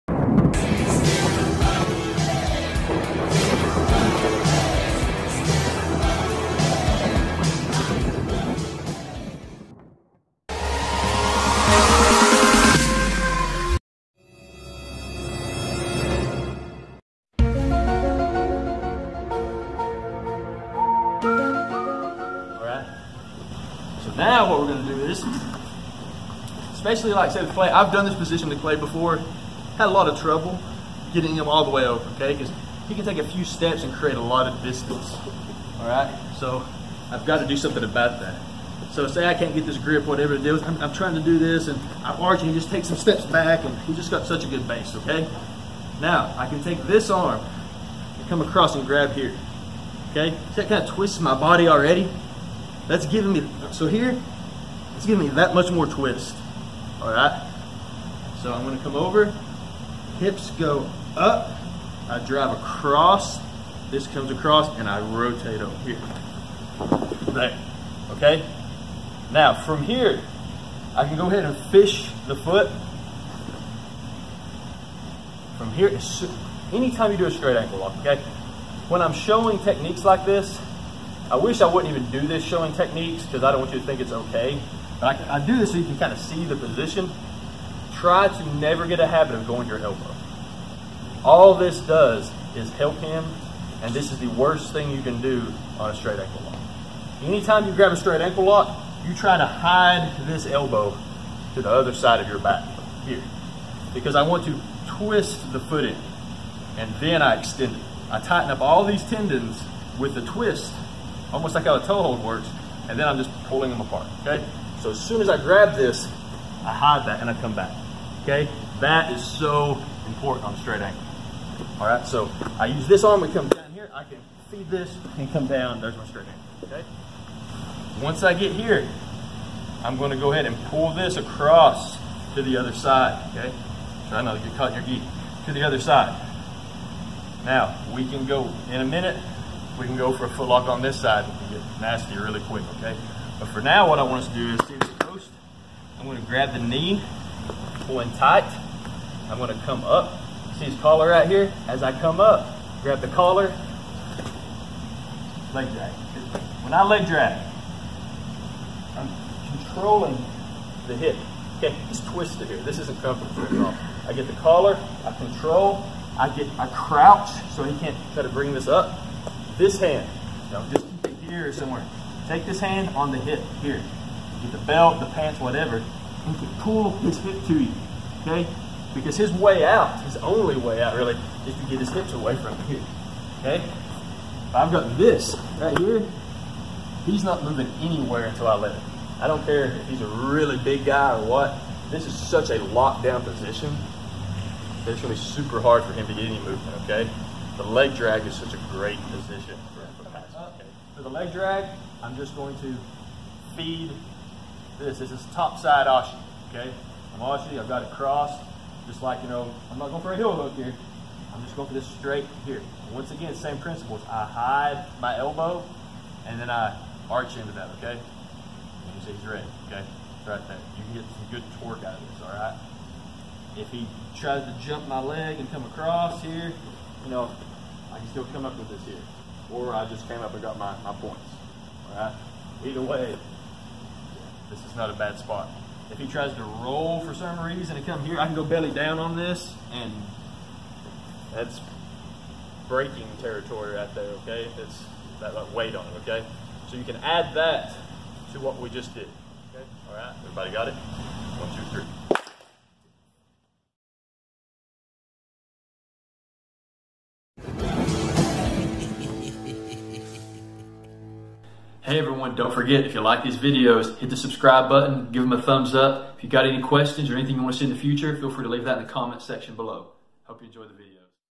X-Guard SLX Leg Drag/Ankle Lock
Single leg X to leg drag or top side ankle lock